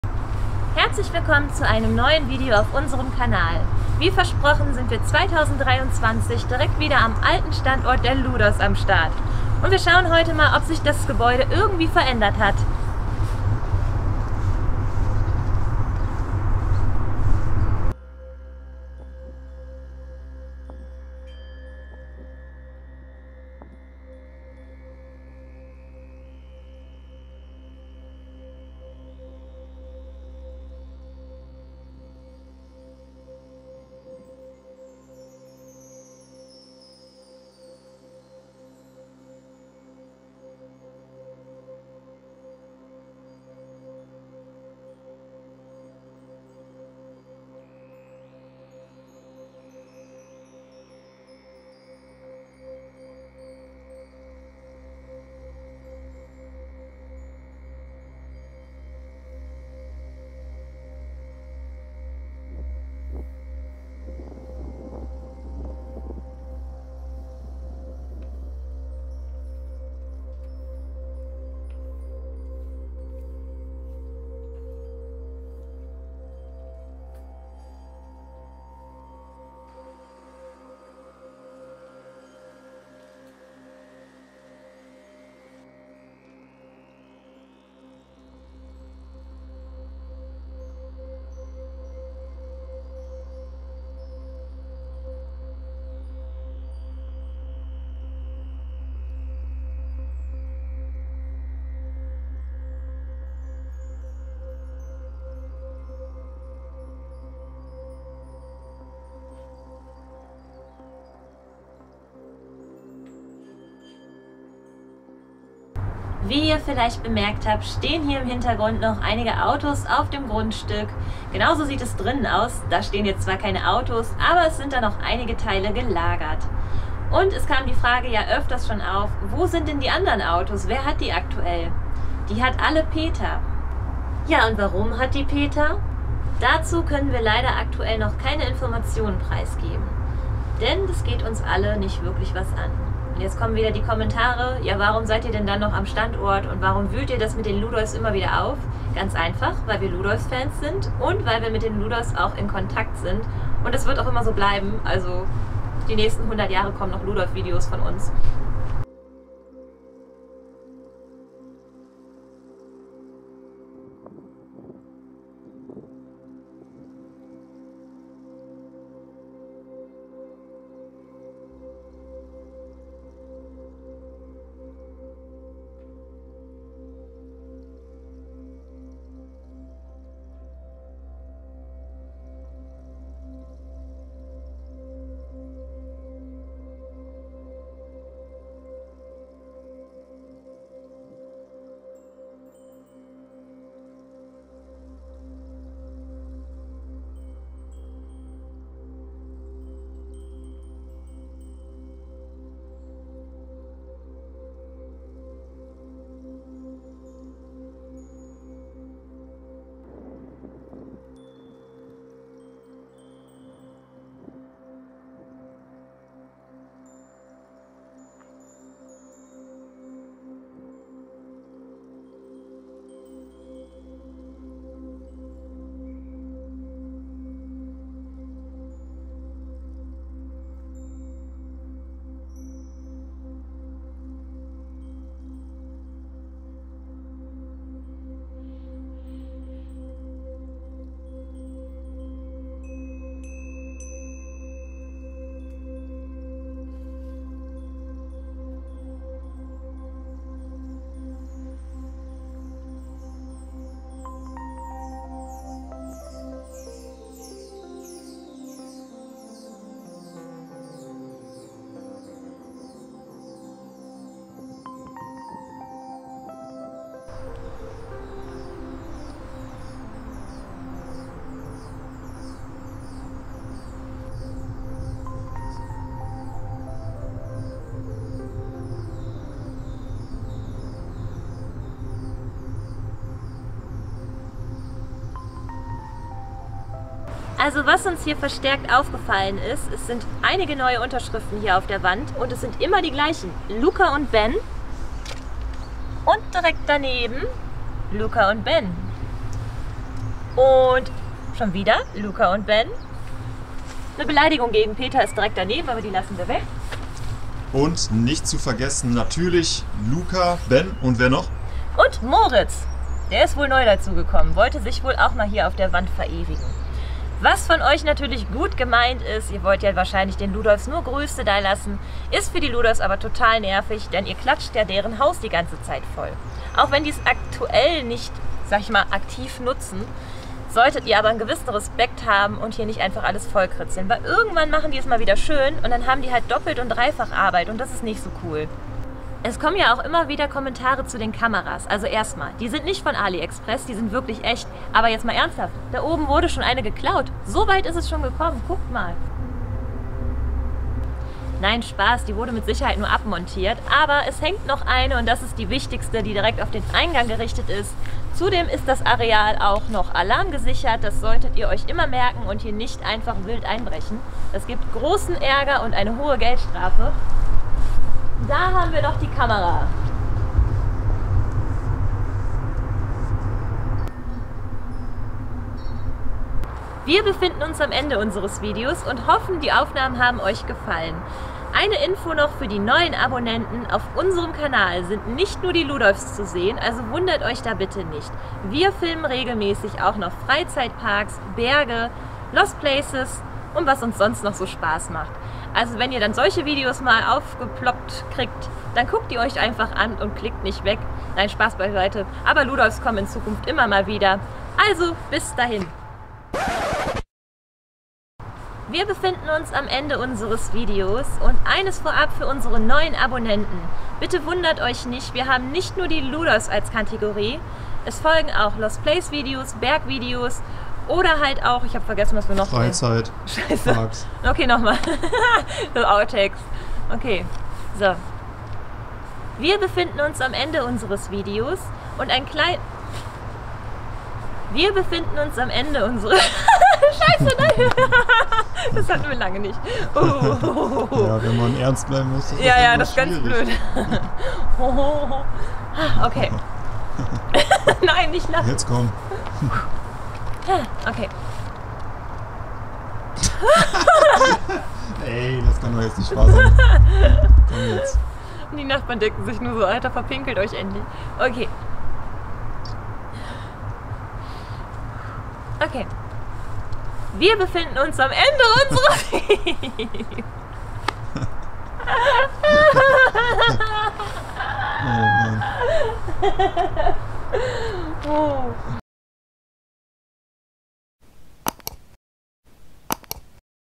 Haus der Ludolfs 2023 | 4 Brüder auf'm Schrottplatz Das verlassene Ludolf Haus heute KEIN Lost Place
Es ist jedes Mal ein trauriges Gefühl wenn man vor Ort ist und an die alten Zeiten denkt, wo die Ludolfs noch dort gelebt und gearbeitet haben.

0:00 - 8:04 Haus der Ludolfs 2023

Fanpost könnt ihr uns gerne an folgendes Postfach schicken:

Adventure Exploring
Postfach 15 01 01
42708 Solingen

Alle Filminhalte sind Eigentum
von "Adventure Exploring"